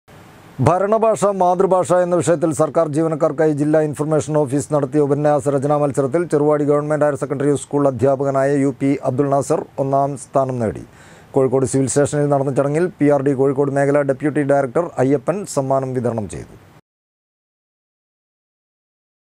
ചെറുവാടി ഗവ.ഹയർ സെക്കണ്ടറി സ്കൂൾ അധ്യാപകനായ യു. പി. അബ്ദുൽ നാസർ ഒന്നാം സ്ഥാനം നേടി
ഭരണ ഭാഷ മാതൃഭാഷ എന്ന വിഷയത്തിൽ സർക്കാർ ജീവനക്കാർക്കായി ജില്ലാഇൻഫർമേഷൻ ഓഫിസ് നടത്തിയ ഉപന്യാസരചന മത്സരത്തിൽ
ചെറുവാടി ഗവ.ഹയർ സെക്കണ്ടറി സ്കൂൾ അധ്യാപകനായ  യു. പി. അബ്ദുൽ നാസർ ഒന്നാം സ്ഥാനം നേടി. കോഴിക്കോട് സിവിൽ സ്റ്റേഷനിൽ നടന്ന ചടങ്ങിൽ PRD കോഴിക്കോട് മേഖല ഡെപ്യൂട്ടി ഡയരക്ടർ അയ്യപ്പൻ സമ്മാനം വിതരണം ചെയ്തു.